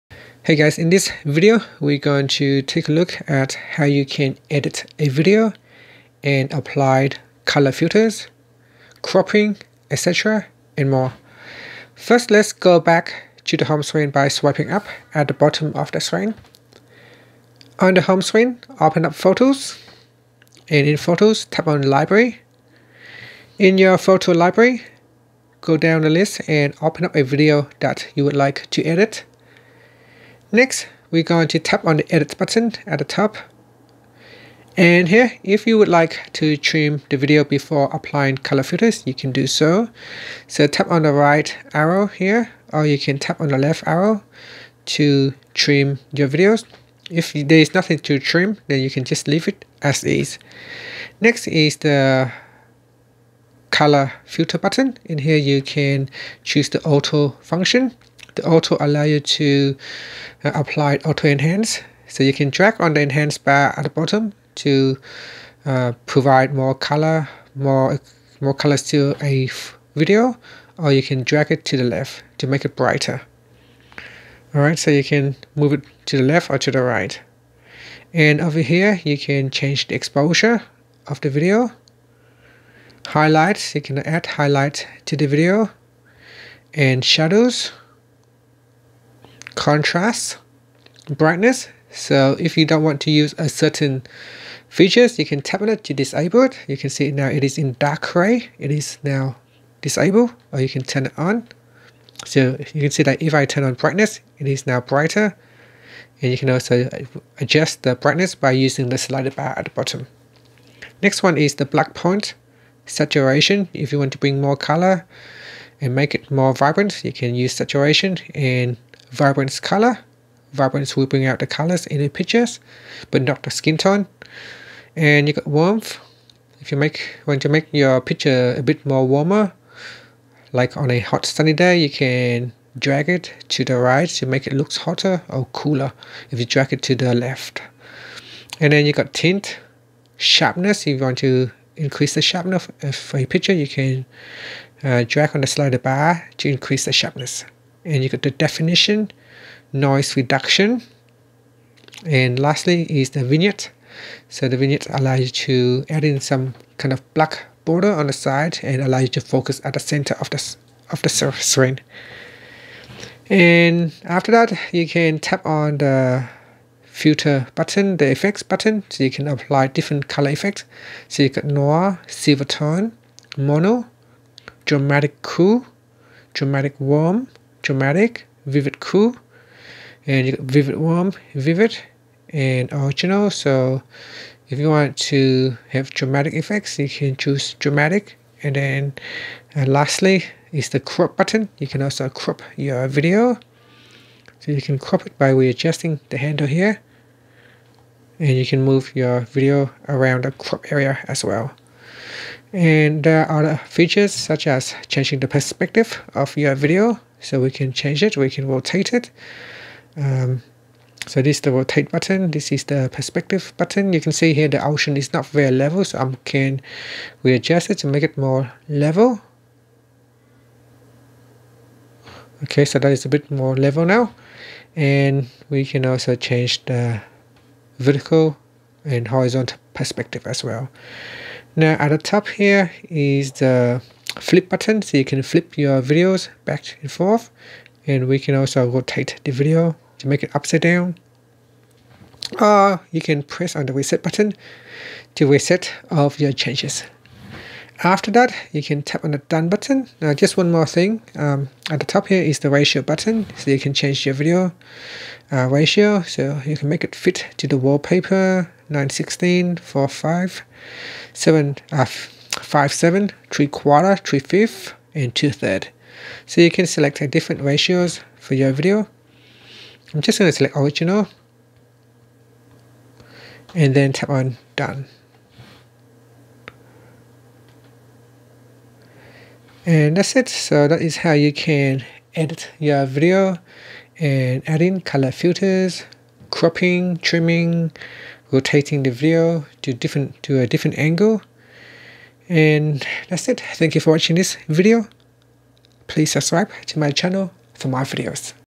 iPhone 14's/14 Pro Max: How to Edit A Video And Apply Color Filters/Trimming/Cropping and More
Learn how you can edit a video and apply color filters, trimming, cropping, and more on the iPhone 14 / iPhone 14 Pro/14 Pro Max/ iPhone 14 Plus.